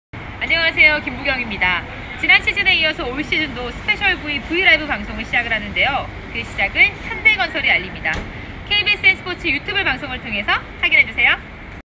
19-20시즌 스페셜V- V라이브 올 시즌도 콜?!
19-20시즌 스페셜V- V라이브 [ 여자배구 - 현대건설 편]
10/16 오후 1시 !
KBSN SPORTS 유튜브 라이브 방송을 통해  확인하세요^^